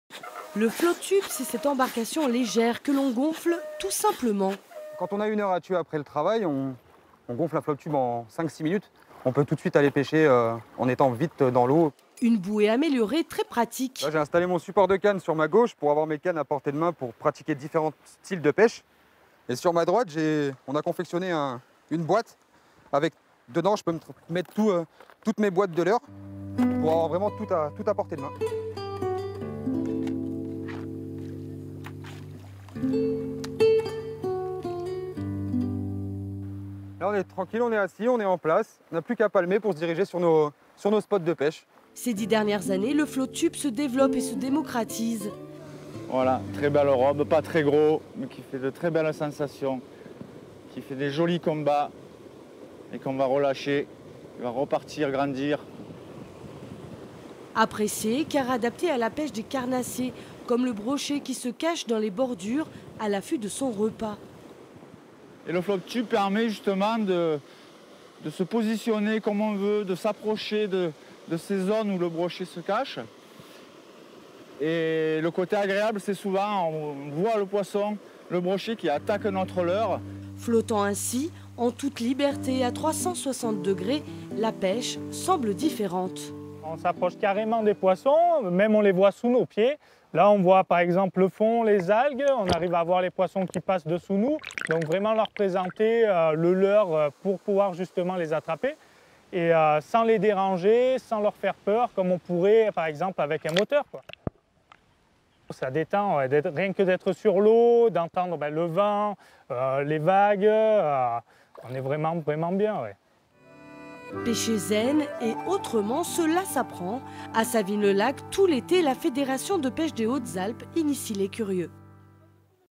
Pêche en "Float tube"
Vous les verrez de plus en plus flotter, les pêcheurs... Ces dernières années, une pratique de la pêche en "Float tube" se développe dans les Hautes-Alpes.

Intervenants :
Romain Anseignat, Pêcheur en float tube
Guy Calvet, Garde pêche, Fédération de pêche des Hautes-Alpes
Kévin Becq,Pêcheur en float tube